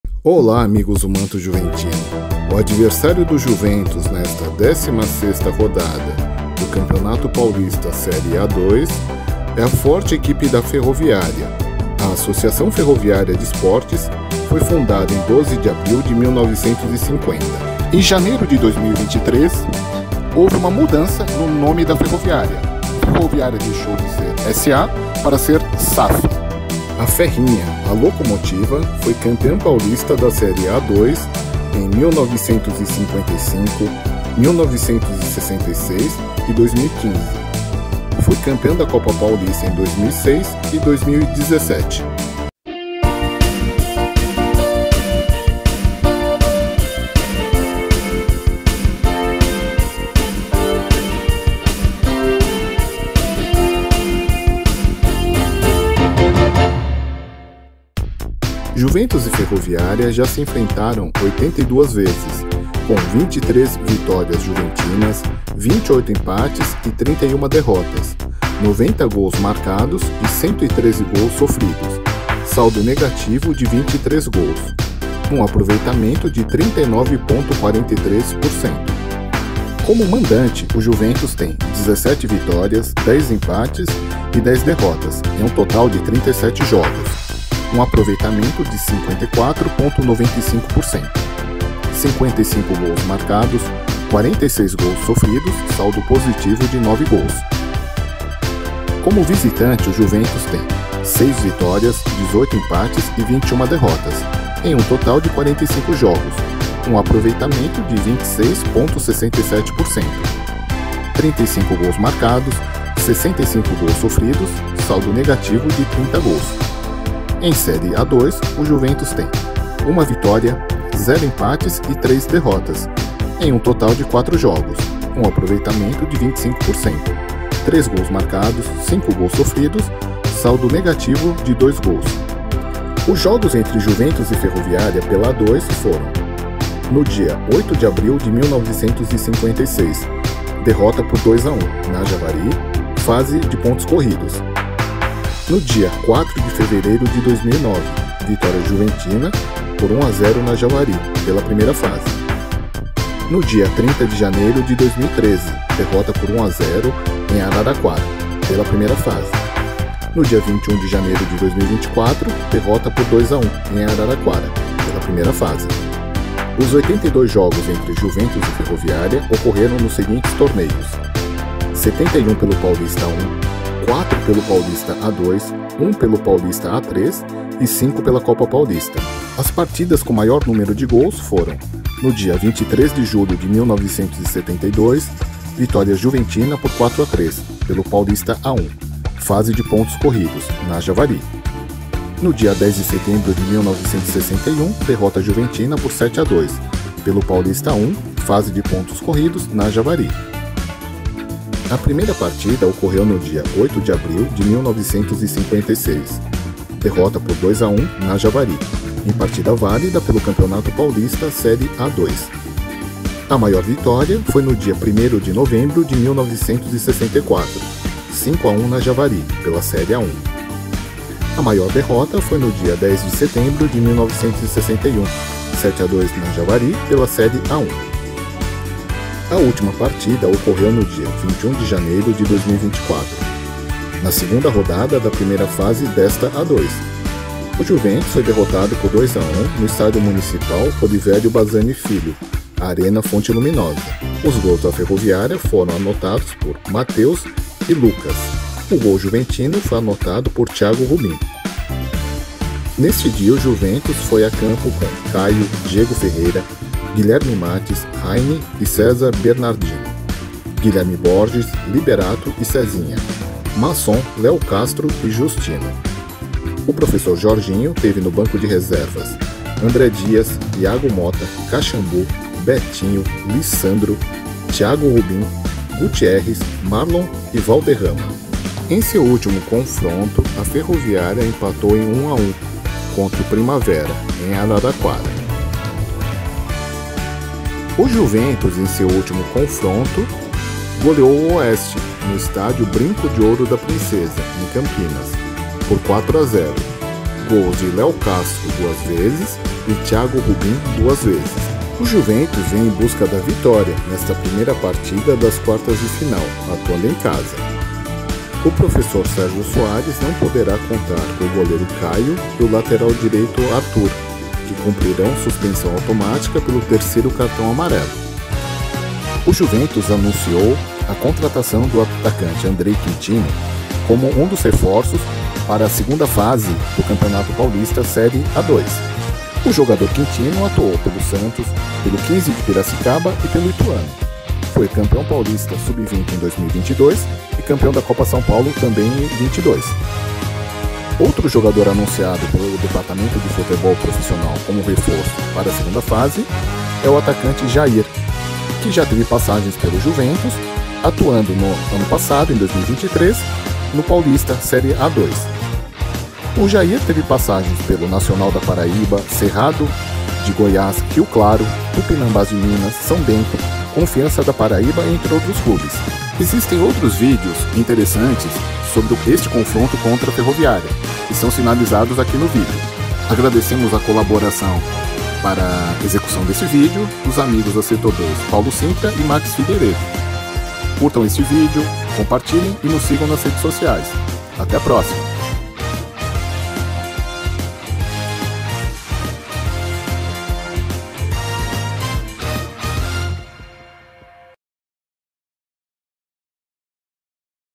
PRÉ-JOGO | JUVENTUS X FERROVIÁRIA | QUARTAS SÉRIE A2 2024
A Série A2 entrou na fase de mata-mata. Trazemos hoje os números, dados, estatísticas e informações sobre o retrospecto do confronto entre Juventus e Ferroviária e notícias da preparação dos clubes.